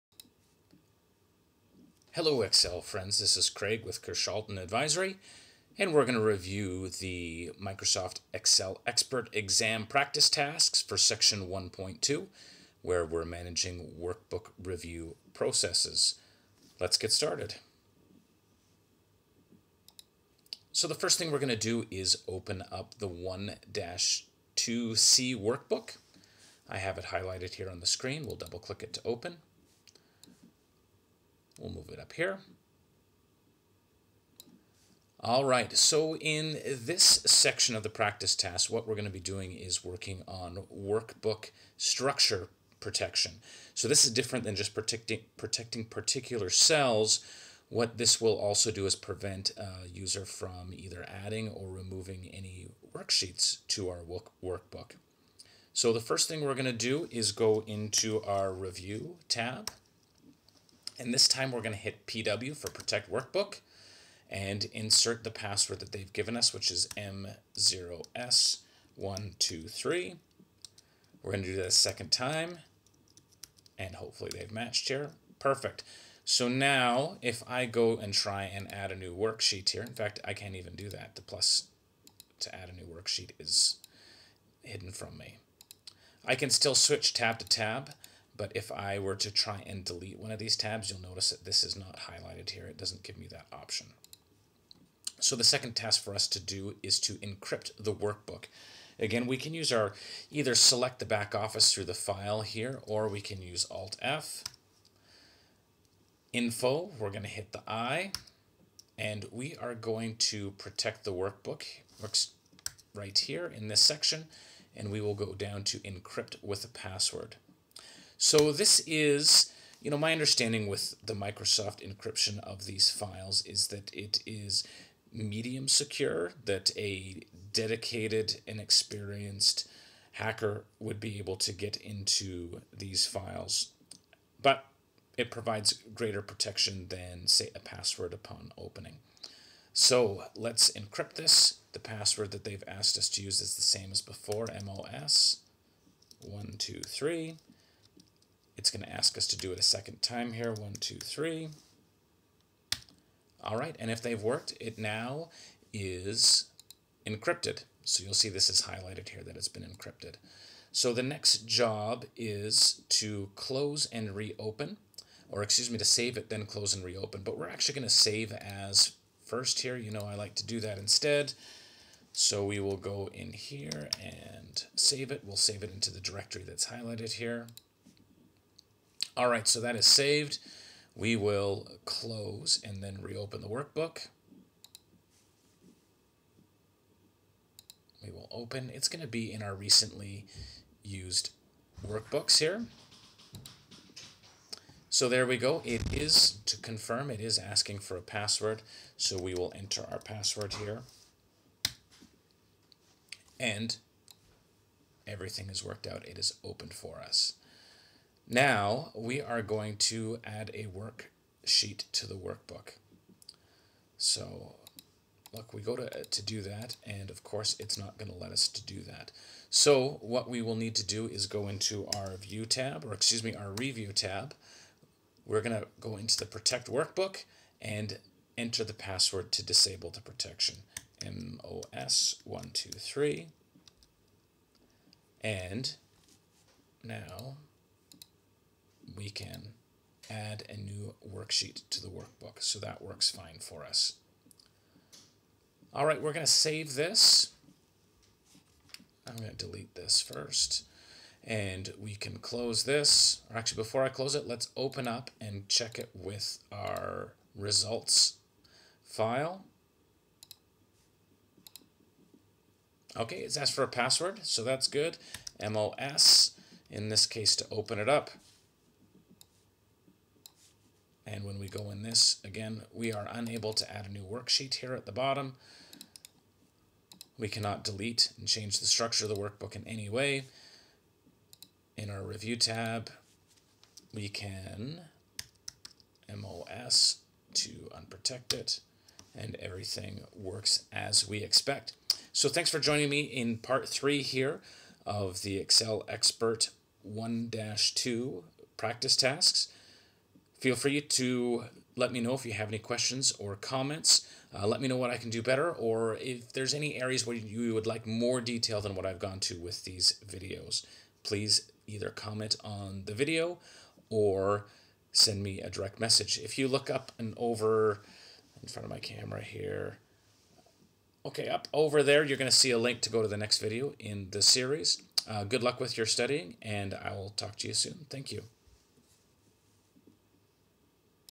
Obj. 1-2 (3/5) Manage Workbook Review 77-728 MOS Excel 2016 Expert Tutorial
Pass the Excel Expert Exam with this tutorial of the practice tasks & Excel tips covering the Microsoft Exam # 77-728 Microsoft Excel Expert (v2016) Study Guide.

Objective 1-2 'Manage Workbook Review.'

This video covers the 3rd section of practice tasks:
 - Protecting workbook structure;
 - Encrypting a workbook; and,
 - Using a password to modify a protected workbook.